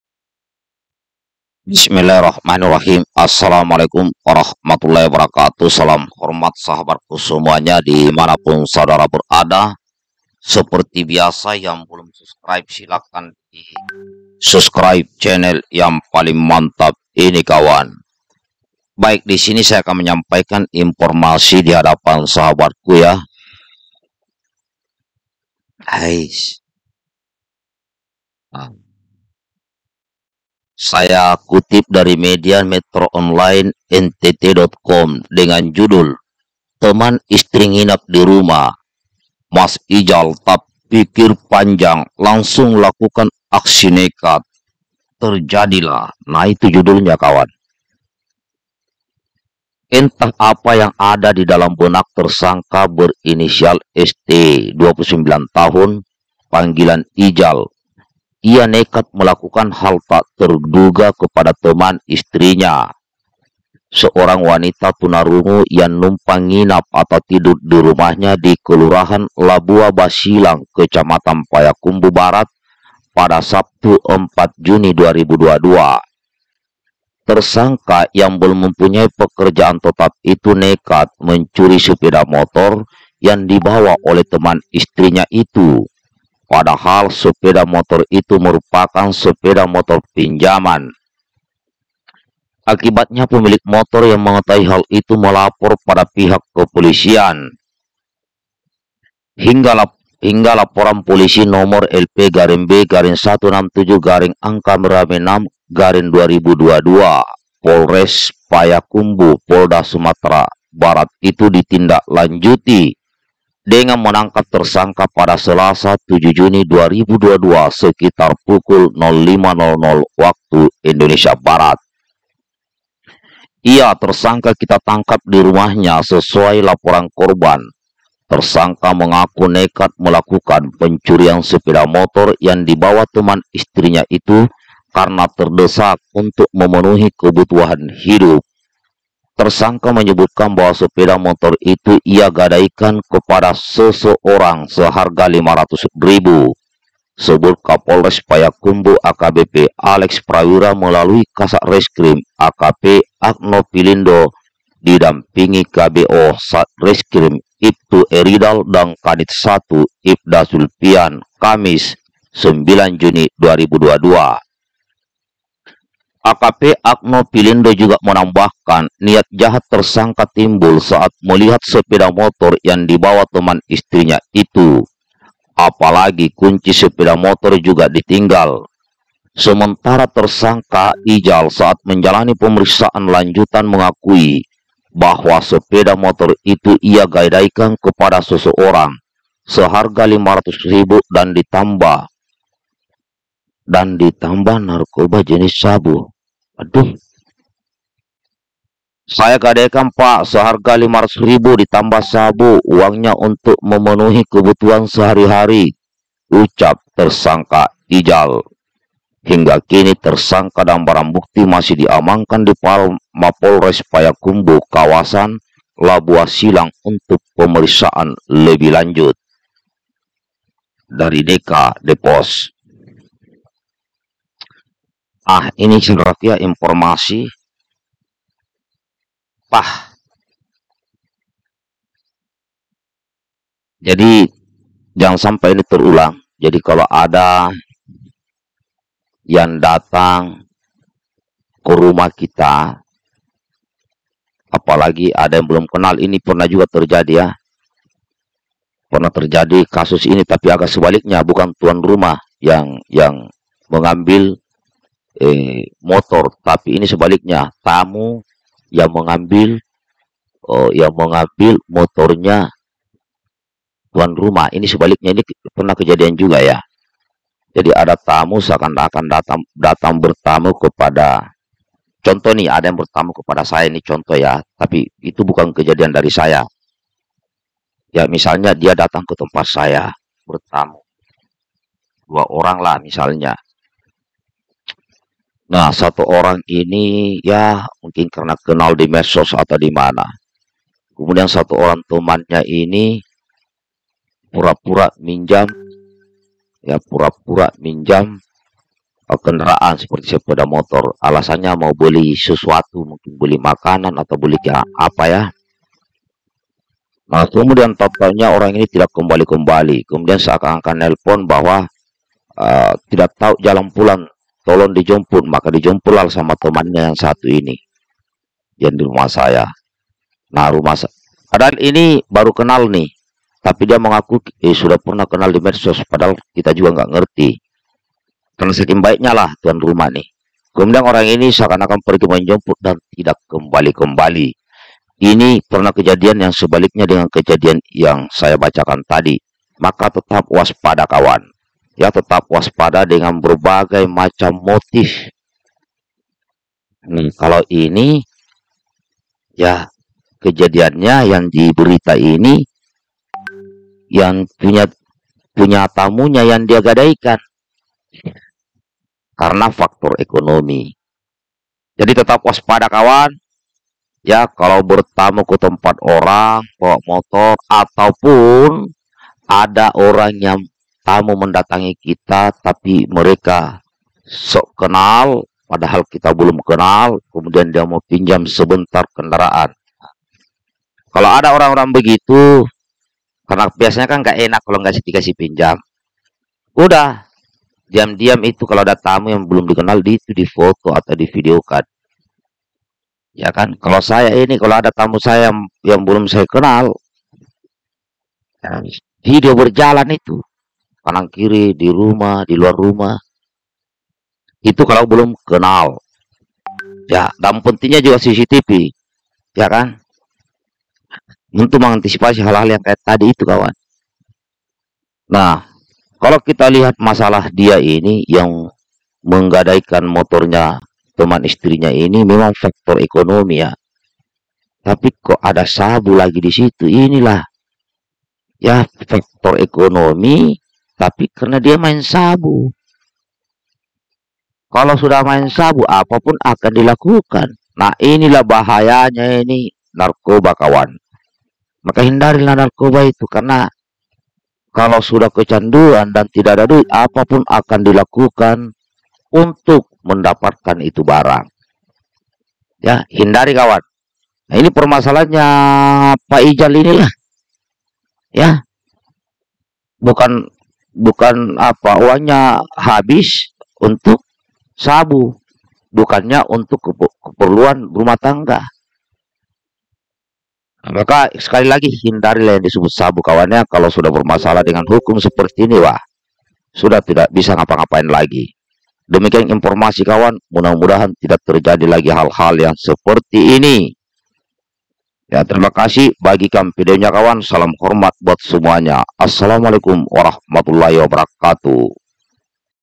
HEBOH. Teman Istri Nginap Dirumah, Pria ini Tak Pikir Panjang. Lakukan Aksi Nekat,AKHIRNYA DITANGKAP
Aamiin.

TTD